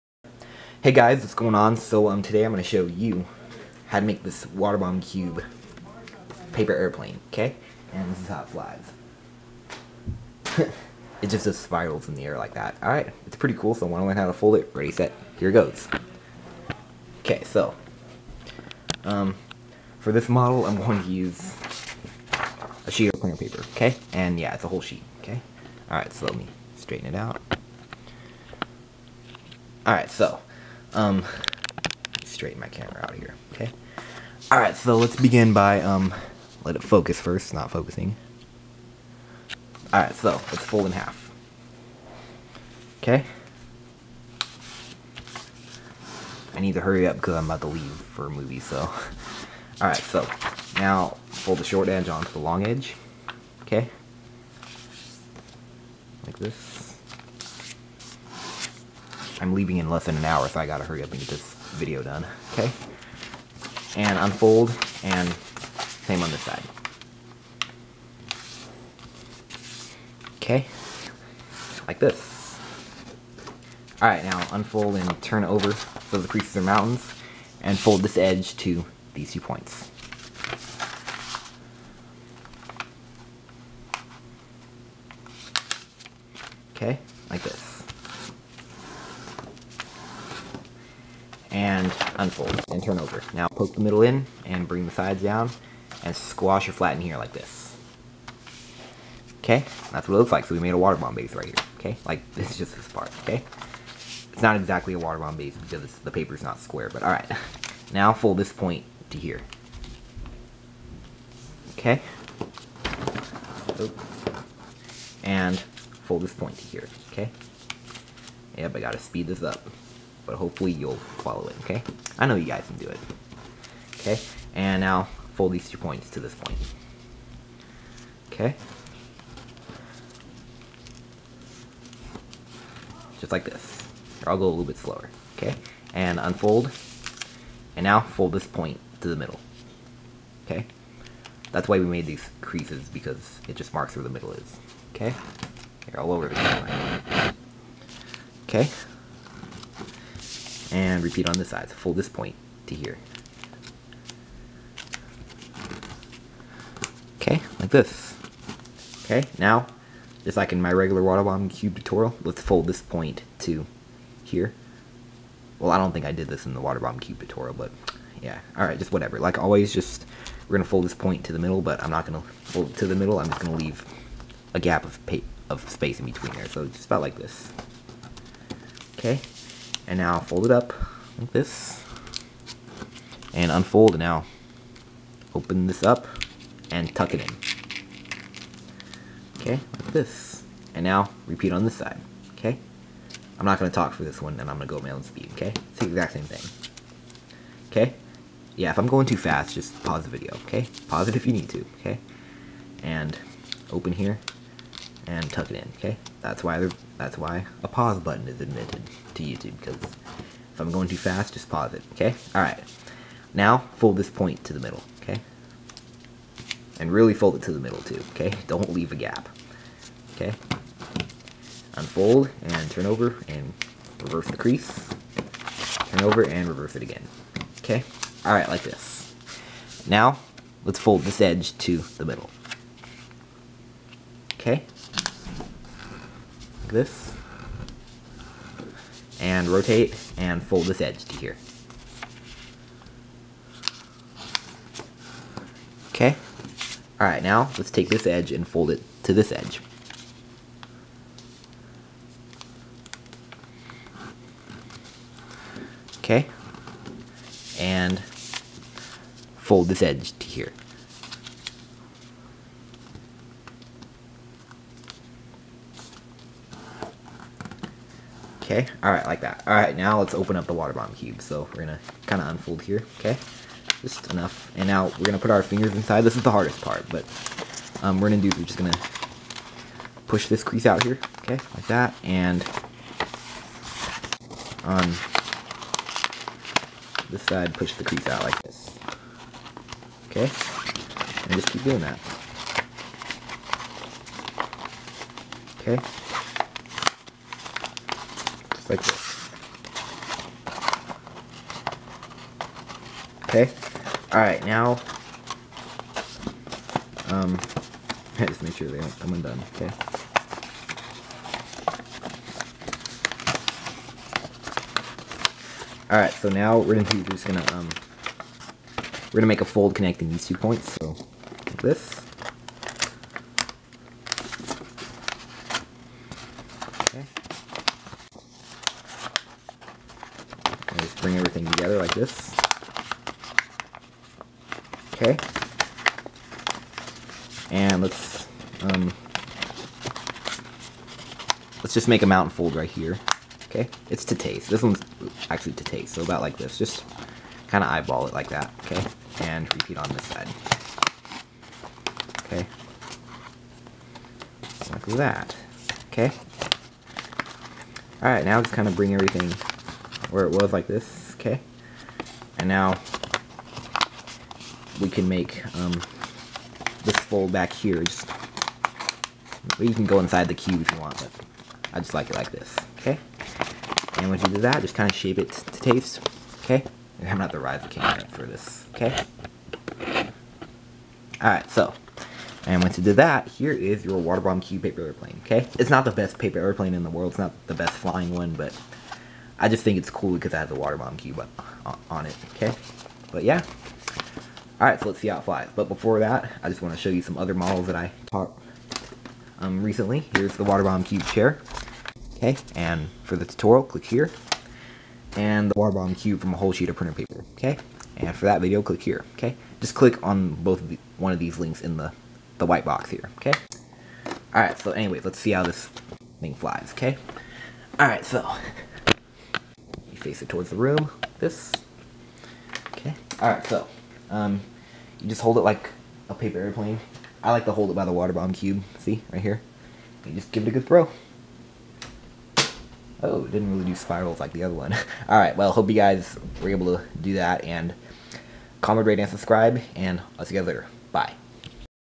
Waterbomb Cube Paper Airplane Tutorial
How to make the waterbomb cube paper airplane.